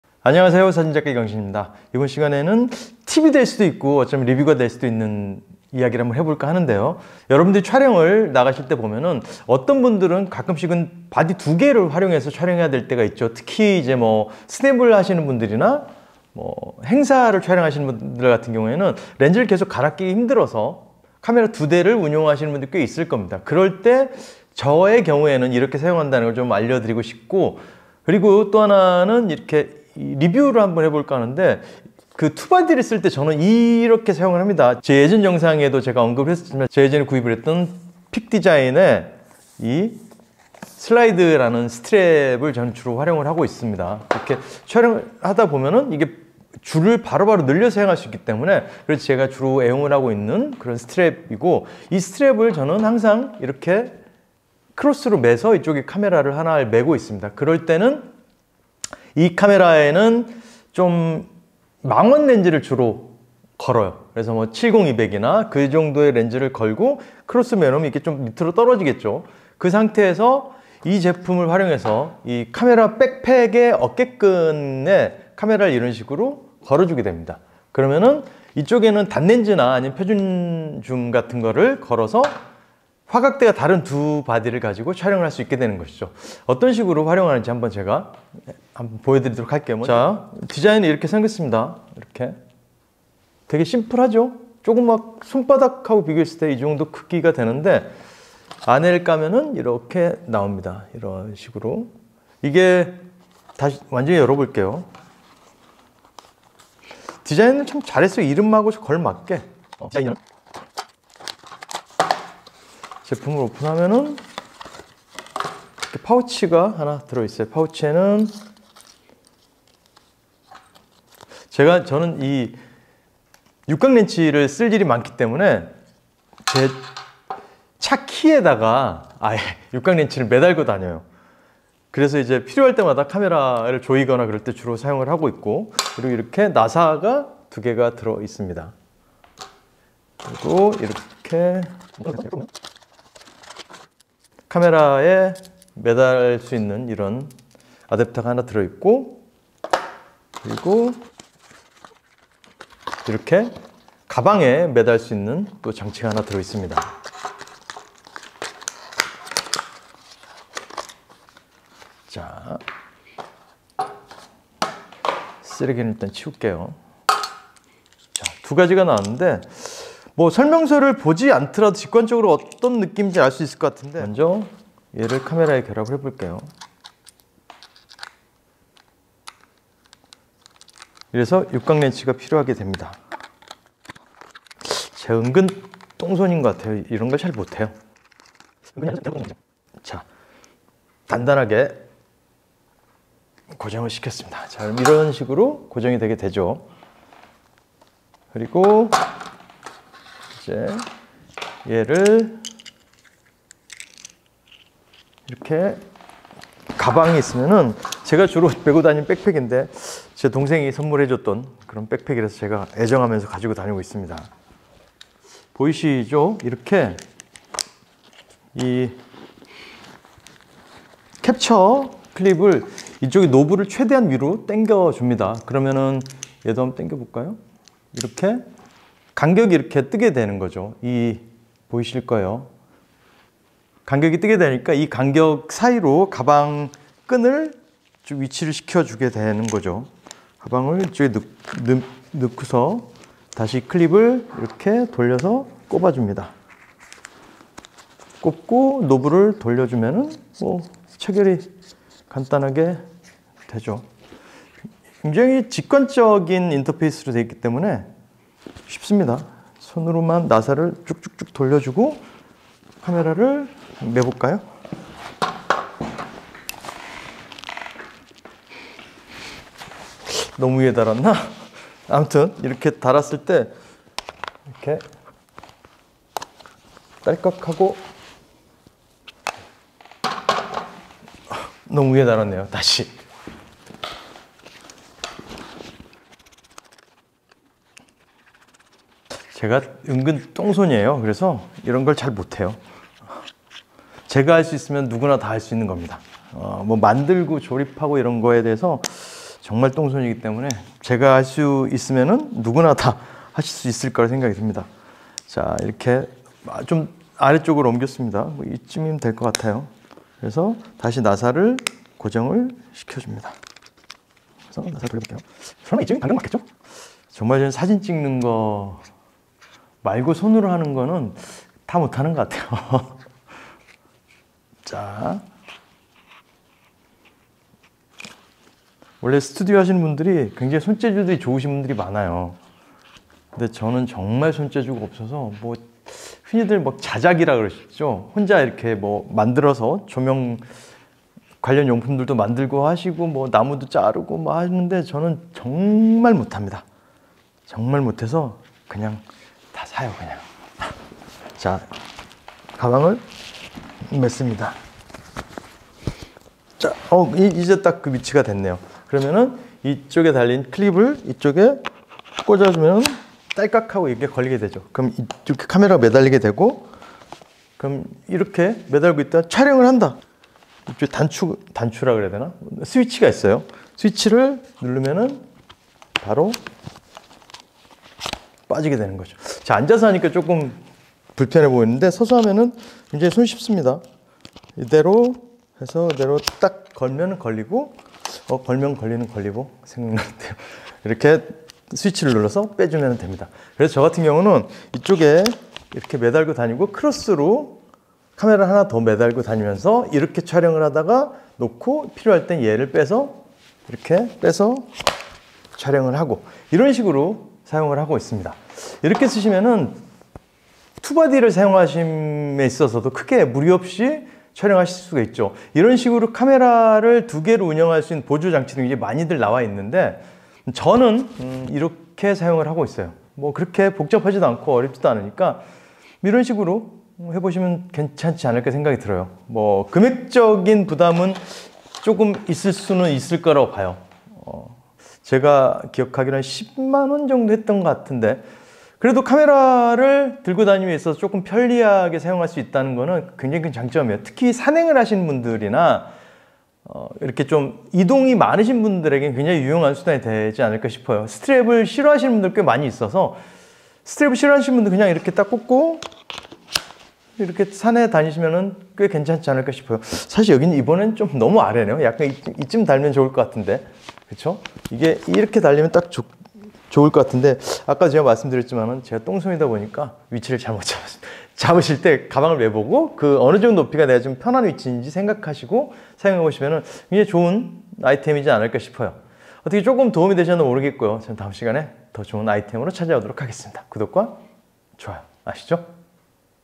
등산을 하면서 촬영하고 싶거나 카메라 2대를 운용하고 싶다면? feat.픽디자인 캡쳐 v3
꼭 카메라 2대를 사용하지 않더라도 카메라 스트랩에 카메라를 매달고
다니면 좀 걸리적 거리고 귀찮은 상황이 생길때가 많습니다.
특히 산행을 함에 있어서 더욱 그런 상황이 발생하게 되죠 그럴때 저는 이렇게
어깨끈에 카메라를 장착해서 산행을 하다가 필요할때 빼서 촬영을 하곤 합니다.
또한 가끔 카메라 2대를 사용하시는 분들이나 렌즈 교환이 귀찮으신 분들은
이 조합 추천 드립니다.
크로스로 스트랩을 이용해서 카메라를 하나 장착하시고
백팩 어깨끈에 이런식으로 카메라를 장착하시면 번거롭지 않게
다양한 화각으로 촬영을 할 수 있답니다.
이 방법 말고도 다양한 방법들이 있을 수 있지만 저는 이런 방법으로
촬영을 하고 있어서 공유해 봅니다~^^